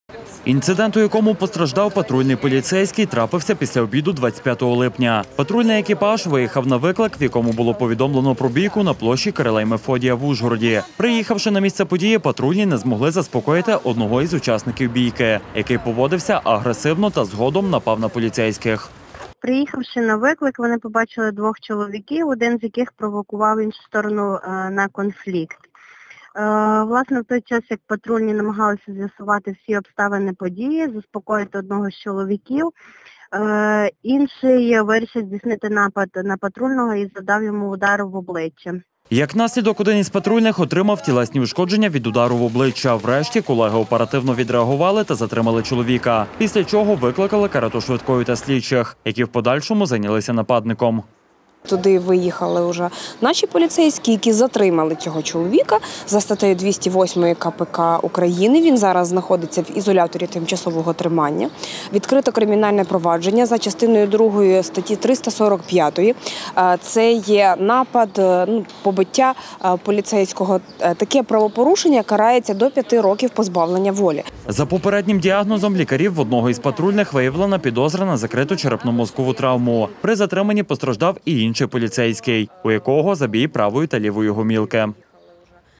Напад на патрульного
Вчора в Ужгороді було здійснено напад на наряд патрульної поліції. Правоохоронці перебували при виконанні службових завдань та виїхали на виклик. На місці події агресивно налаштований чоловік напав на поліцейського, нанісши тілесні ушкодження. Інцидент, у якому постраждав поліцейський патрульної поліції, трапився після обіду 25 липня. Патрульний екіпаж виїхав на виклик, в якому було повідомлено про бійку на площі Кирила і Мефодія в Ужгороді. Приїхавши на місце події, патрульні не змогли заспокоїти одного із учасників бійки, який поводився агресивно та згодом напав на поліцейських.